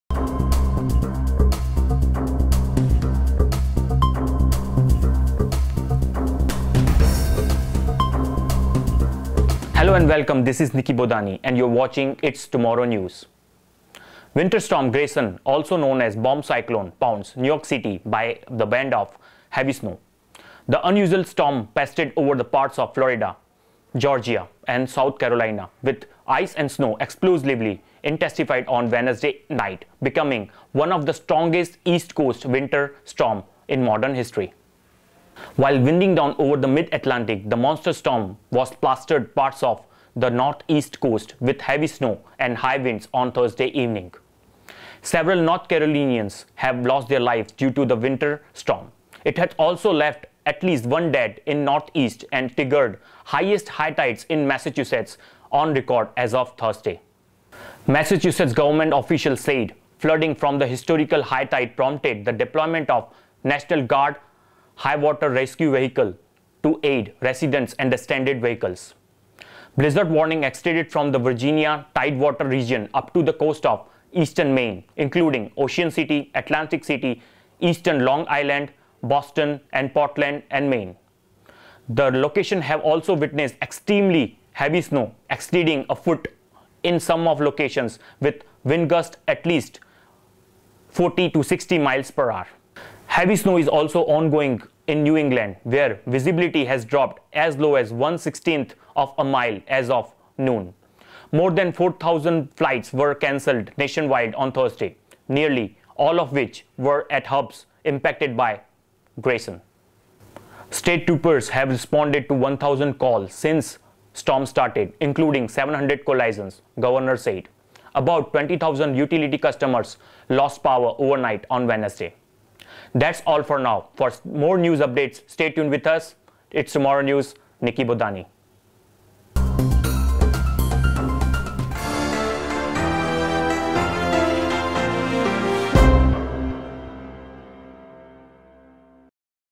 Winter Storm Grayson
Winter Storm Grayson also known as “bomb cyclone” pounds New York City by a band of heavy snow. The unusual storm pasted over parts of Florida, Georgia and South Carolina with ice and snow explosively intensified on Wednesday night becoming one of the strongest East Coast winter storms in modern history. While winding down over the Mid-Atlantic, the monster storm was plastering parts of the Northeast coast with heavy snow and high winds Thursday evening.